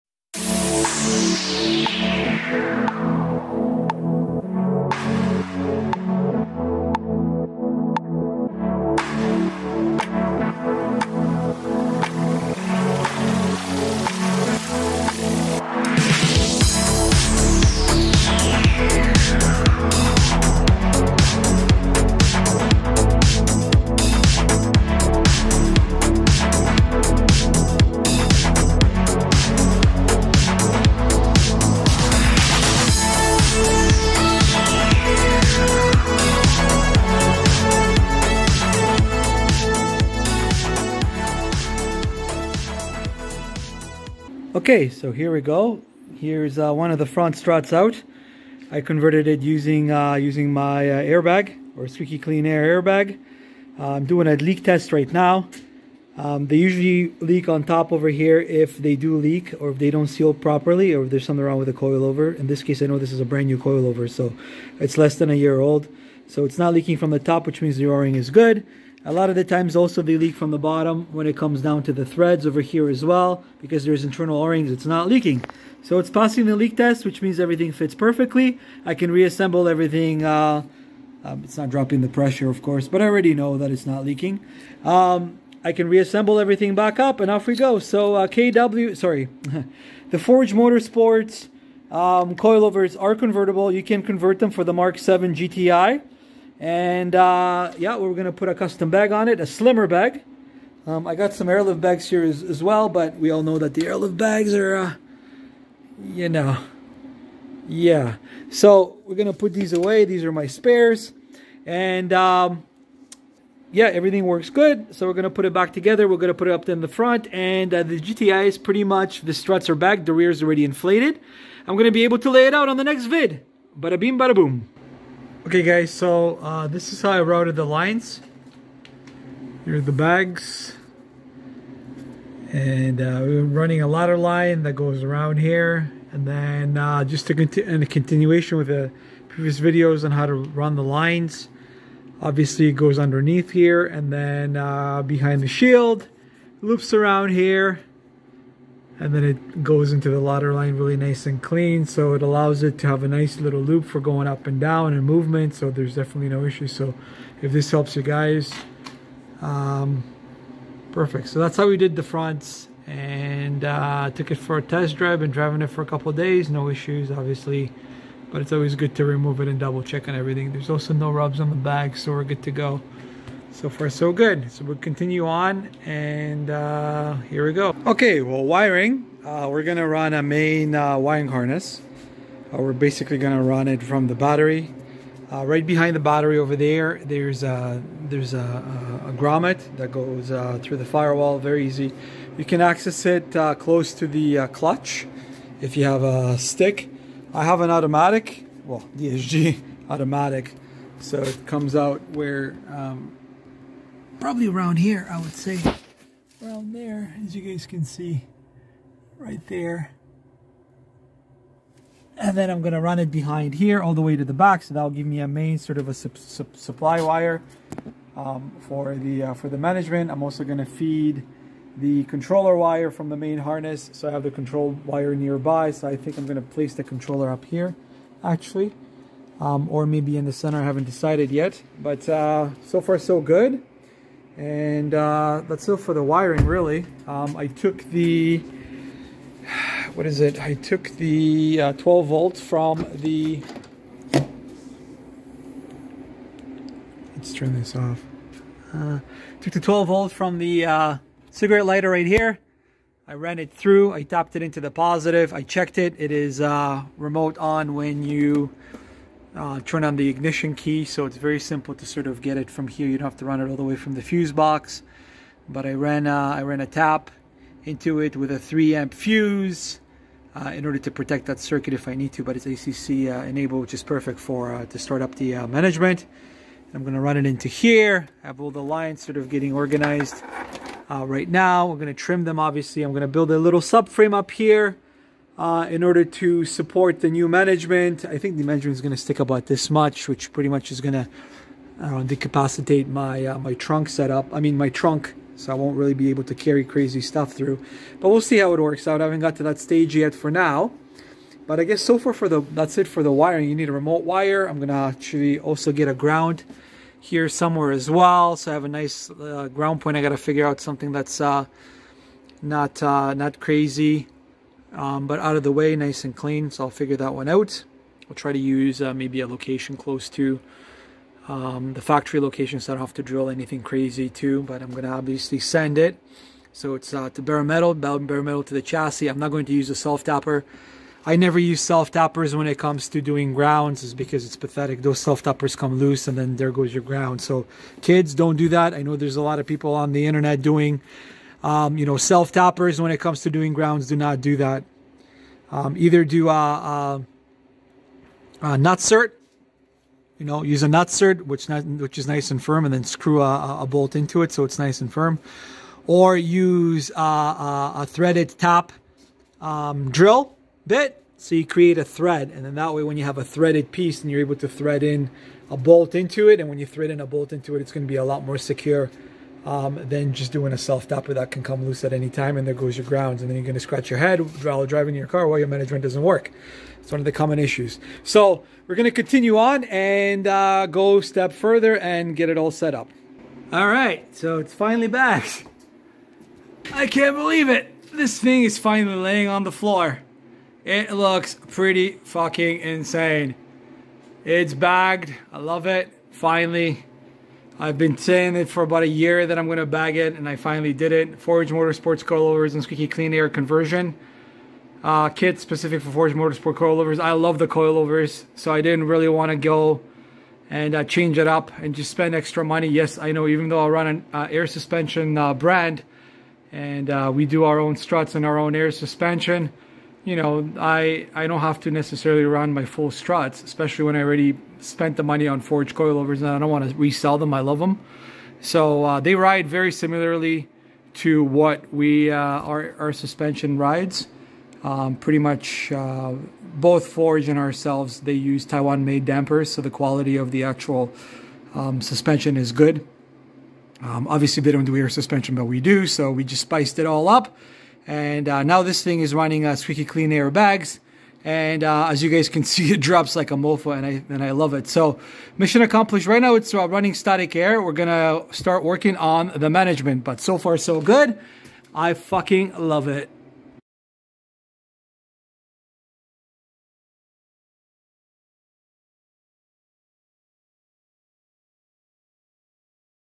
Bagging our Golf MK7.5 GTI Part.3.
Part three of the series where we bag our MK7.5 GTI golf.